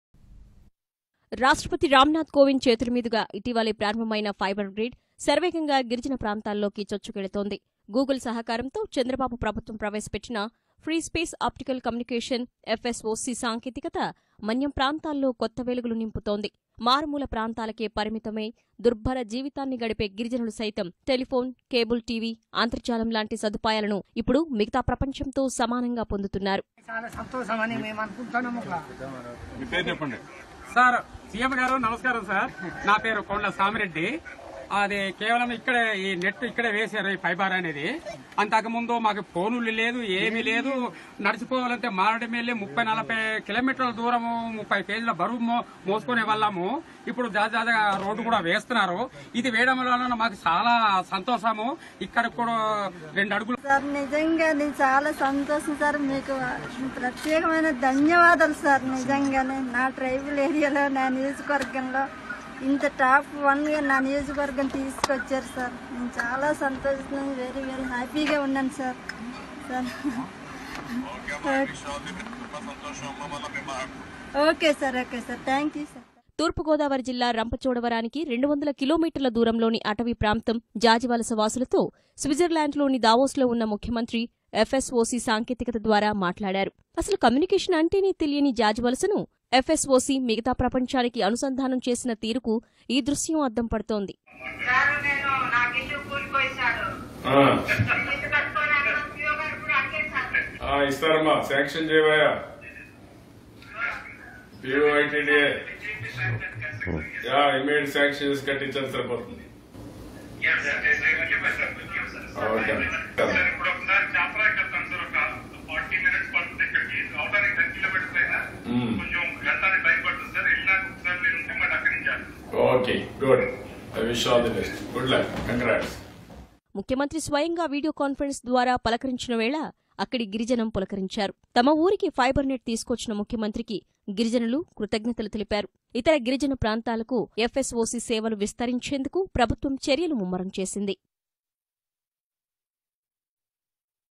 Sri NCBN sir video conference with Jajivalasa villagers, East godavari
తూర్పుగోదావరి జిల్లాలోని వై.రామమవరం మండలంలో ఉన్న మారుమూల గిరిజన గ్రామం జాజివలస. రంపచోడవరం నుంచి దాదాపు 80కి.మీ.ల దూరంలో ఉండే ఈ కుగ్రామానికి ఇప్పటివరకు ల్యాండ్‌ఫోను, సెల్‌ఫోను సదుపాయమే లేదు. ఎందుకంటే ఇది కొండకోనల మధ్య ఉంది. అలాంటి గ్రామానికి మంగళవారం తొలిసారి ఫోన్‌, ఇంటర్నెట్‌, కేబుల్‌ టీవీ సదుపాయాలొచ్చాయి. అసలు ఫోను సదుపాయమే లేని గ్రామస్థులకు ఫోను రావడమే అద్భుతమనుకుంటే, ఆ ఫోనులో నేరుగా రాష్ట్ర ముఖ్యమంత్రే మాట్లాడితే మహాద్భుతం కదా!
ఇదంతా ఏపీ ఫైబర్‌నెట్‌ సంస్థ, ‘గూగుల్‌ ఎక్స్‌’ సంస్థలు అందించిన ఫ్రీ-స్పేస్‌ ఆప్టికల్‌ కమ్యూనికేషన్‌’ (ఎఫ్‌ఎస్‌వోసీ) పరిజ్ఞానం మహిమ. అంతకుమించిన విషయం ఏమిటంటే ఈ సదుపాయాన్ని ముఖ్యమంత్రి చంద్రబాబు దావోస్‌ నుంచి ప్రారంభించడం. దావోస్ లో ద్వైపాక్షిక సమావేశాలతో తీరికలేకుండా ఉన్న ముఖ్యమంత్రి జాజివలస ప్రజలతో వీడియో కాన్ఫరెన్సు ద్వారా మాట్లాడటం. తనకు ఇల్లు లేదని చెప్పిన మహిళకు అప్పటికప్పుడే ఇల్లు మంజూరుచేయమని ఆదేశించడం. దీన్నంతా కళ్ళకు కడుతున్న వీడియో ఇది.